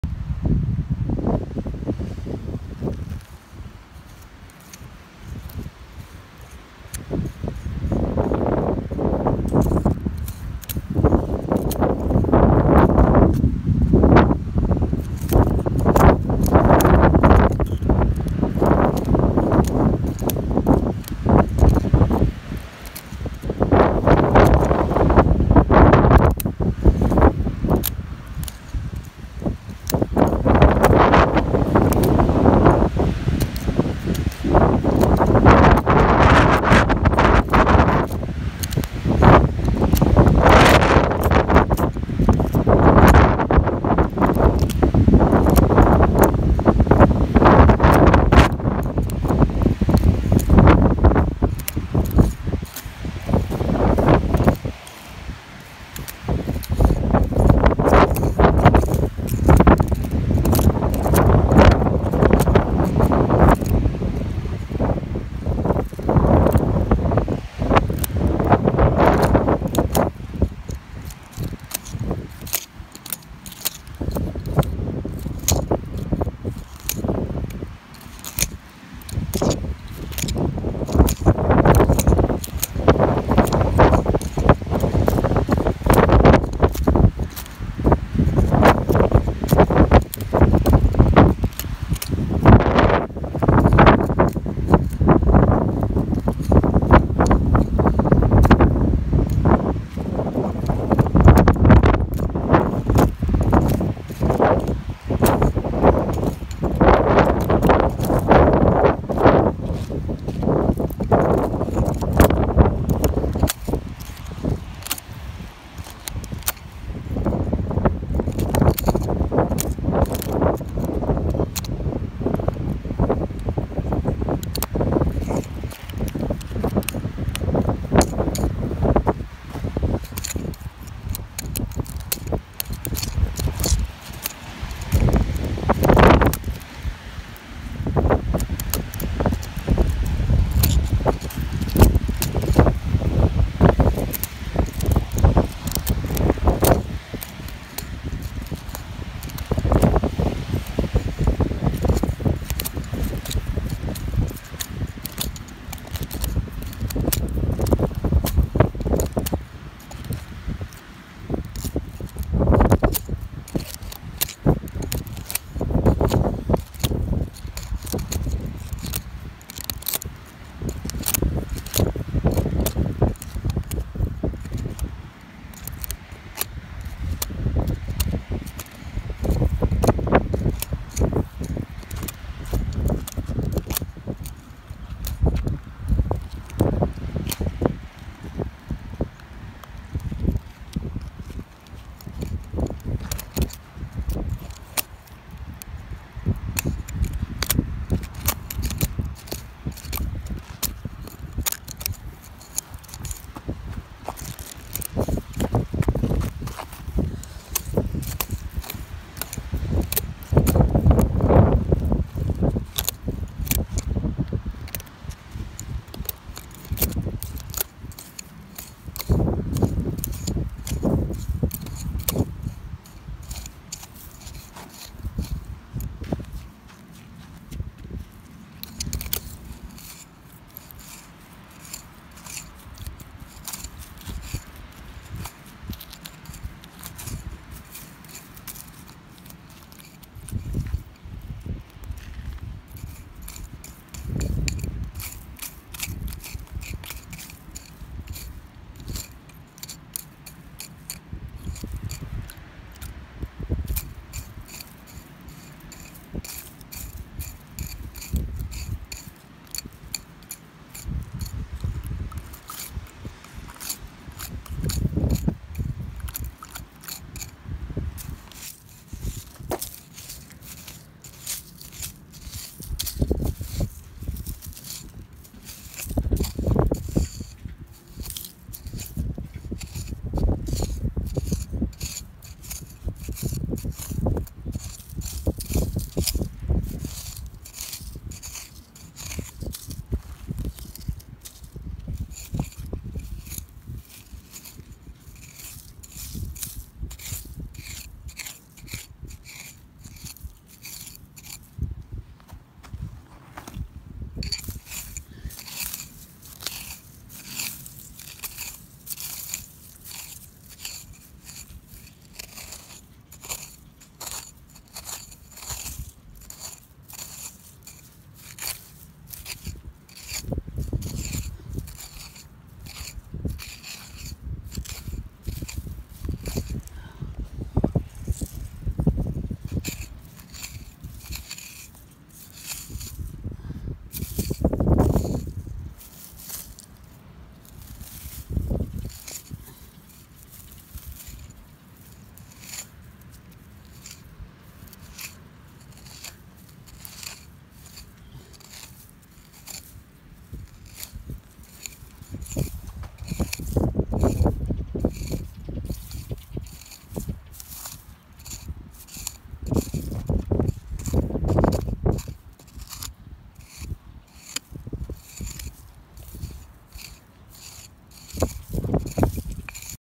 Removing Weeds Around Cactus Plants Using a Screwdriver
Red barrel cactus 0:00
Golden torch cactus 03:50
Opuntia cactus 04:29